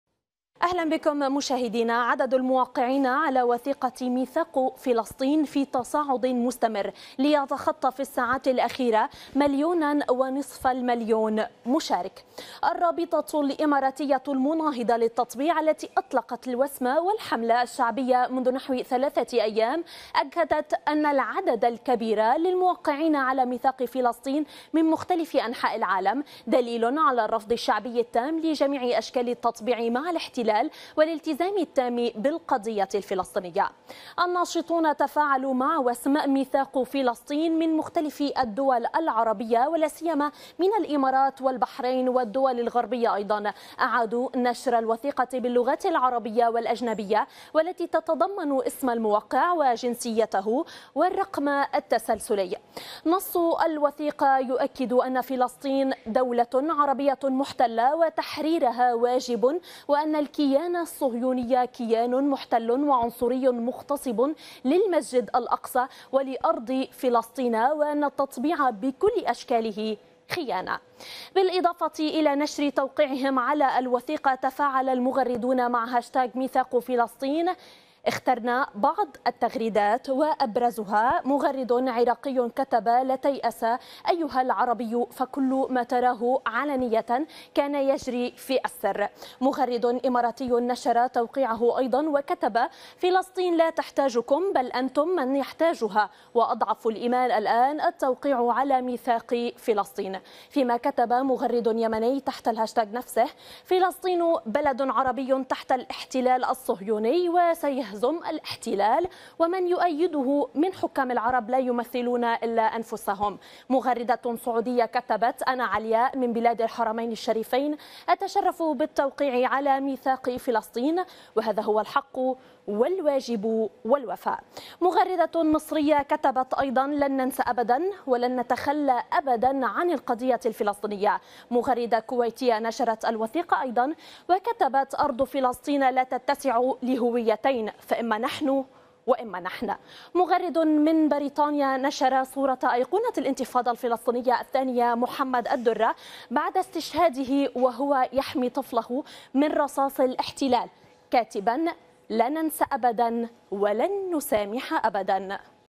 عدد الموقعين على وثيقة "ميثاق فلسطين" في تصاعد مستمر
إعداد: مريم عثمان
تتواصل المواقف الرافضة للتطبيع التي تحفل بها مواقع التواصل الاجتماعي. وفي هذا السياق تخطى عدد الموقّعين على وثيقة ميثاق فلسطين على مواقع التواصل حاجز المليون ونصف المليون موقع من مختلف دول العالم.